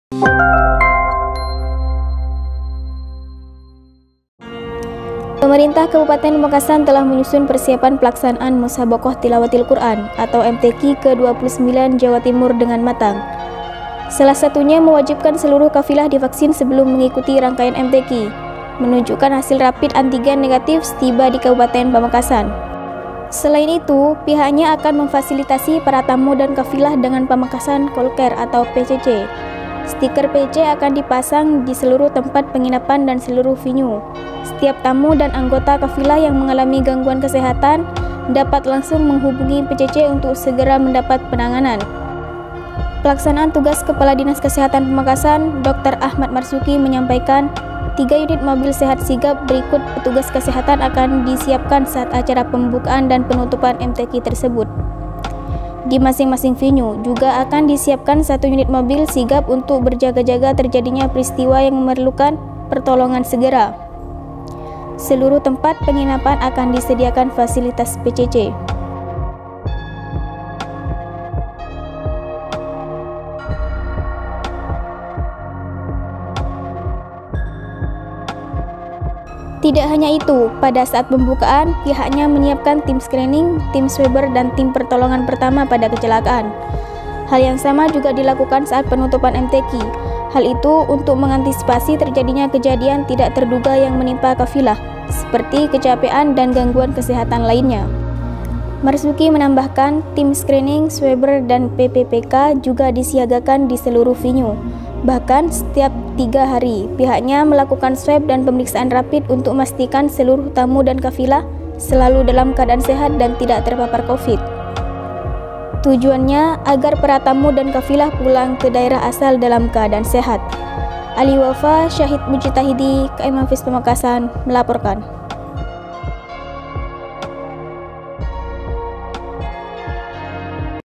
Demi Pelayanan Kesehatan yang Prima untuk Kafilah, Pemkab Pamekasan Siagakan PCC dan Mobil Sigap
Editor+VO : Hafidzatul Qoyyimah